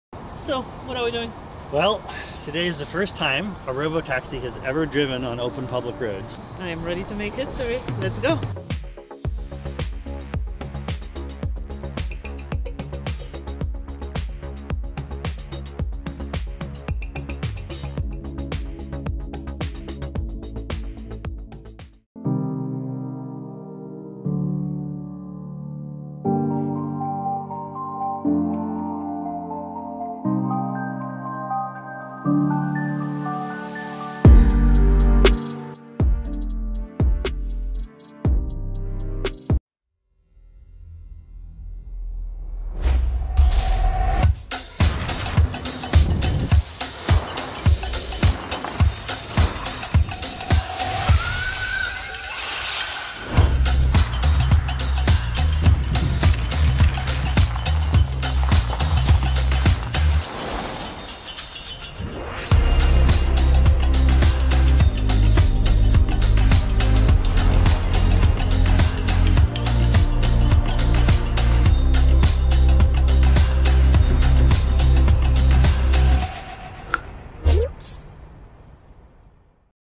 Amazon successfully tested a self driving robo taxi with employees as passengers on a public road
Amazon-owned self-driving startup Zoox has successfully tested a robotaxi with employees as passengers on a public road, bringing it closer to commercial service for the general public